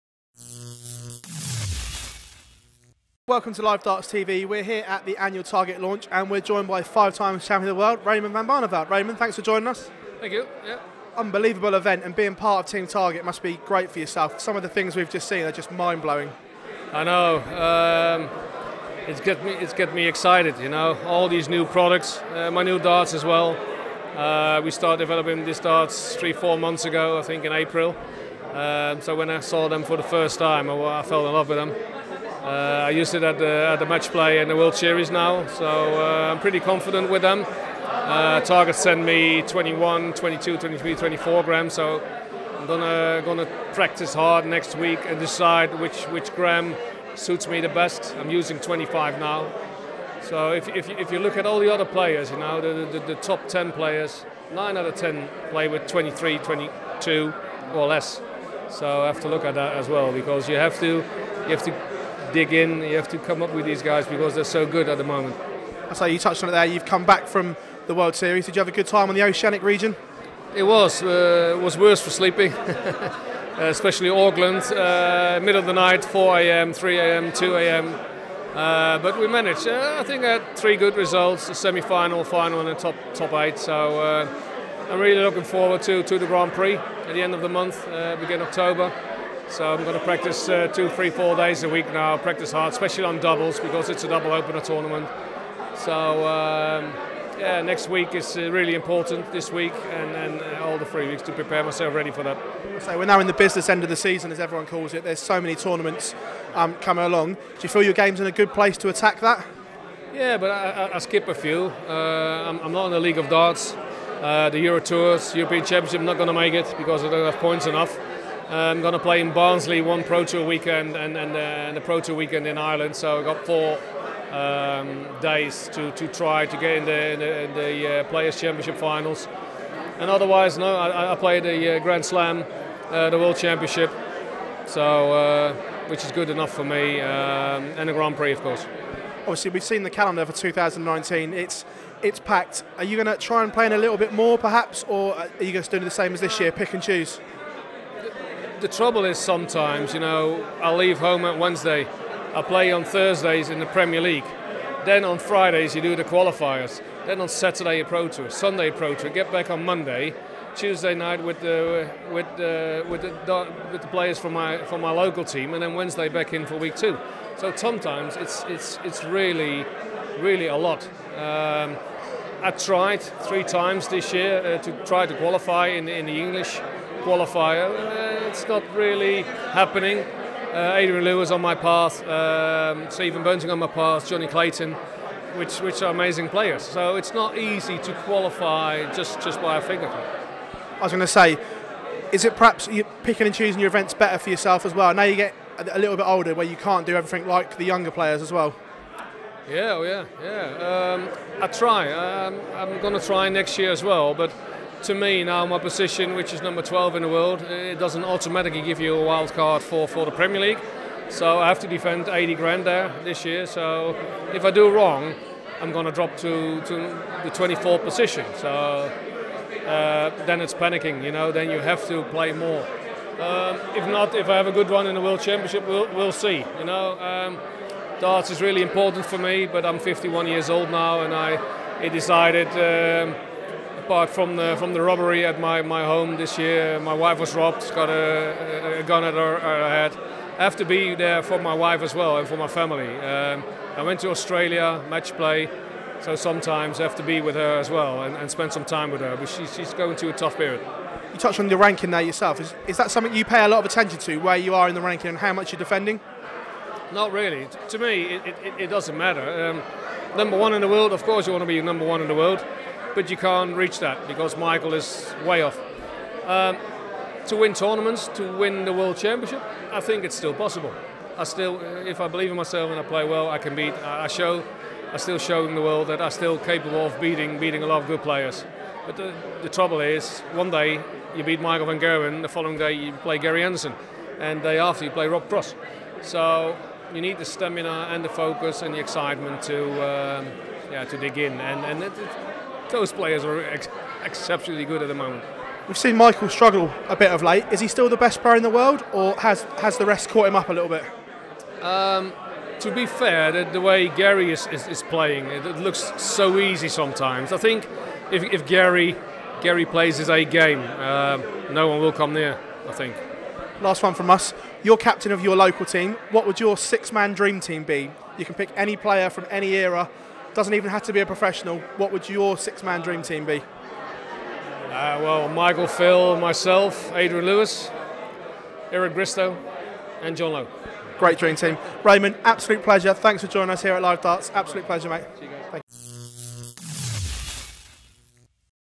Raymond Van Barneveld - “I could drop to 24th position then it’s panic”
Five-time World Champion Raymond van Barneveld reflects on his World Series campaign and reveals his plans for the remainder of the 2018 season.